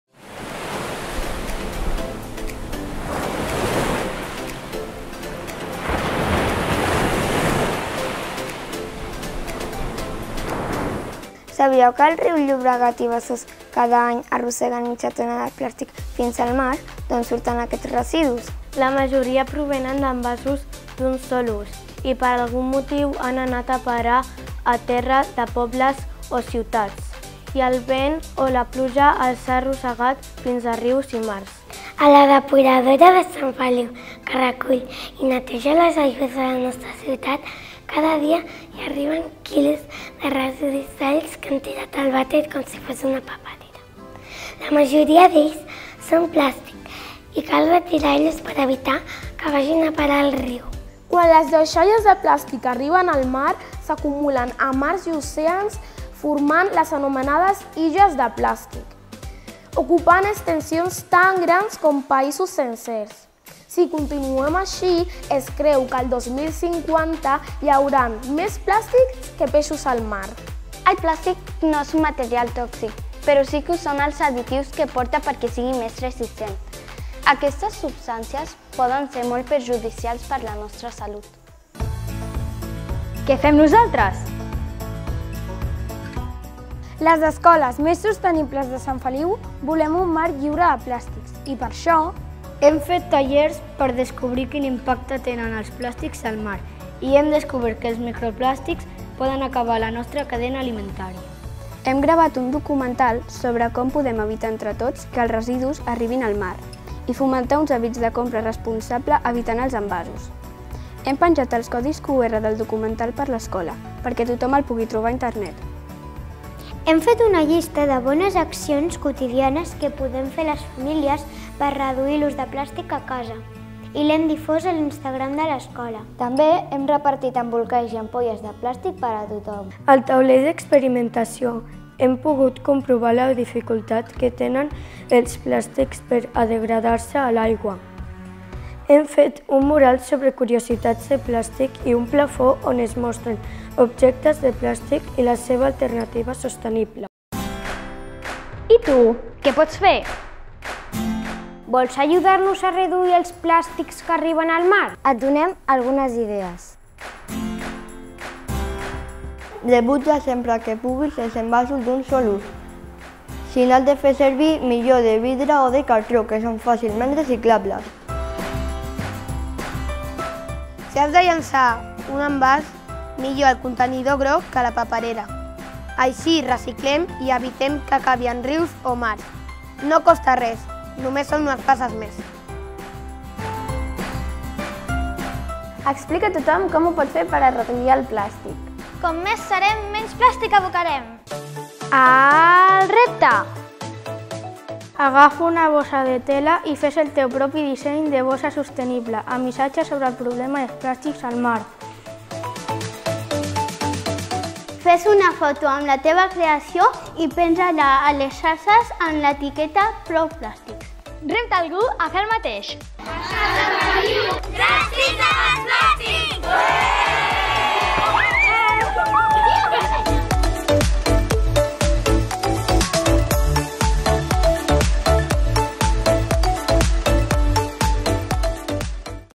Mars de plàstic
Vídeo creat en el marc de la campanya "A Sant Feliu, dràstics amb els plàstics", impulsada per l'Oficina de Serveis Ambientals de l'Ajuntament en col•laboració amb els centres educatius de la xarxa Escoles + Sostenibles i entitats del municipi per conscienciar sobre la importància dels ecosistemes marins.

Aquest projecte conjunt de sensibilització sobre l'impacte dels plàstics s'emmarca dins el Pla de prevenció de residus municipal, i llança reptes a la ciutadania buscant la seva implicació per reduir l'ús dels plàstics a la vida quotidiana. La proposta és alhora l’acció conjunta d’aquest curs de la xarxa Escoles + sostenibles, atès que la trobada anual que realitzen aquestes escoles, la TROCAMBA, no s’ha pogut realitzar amb el format habitual a causa de les restriccions provocades per la pandèmia. Hi ha participat l’Escola Verge de la Salut, el Col•legi Mare de Déu de la Mercè, l’Escola Mestral i l’Escola Arquitecte Gaudí.

A Sant Feliu, dràstics amb els plàstics!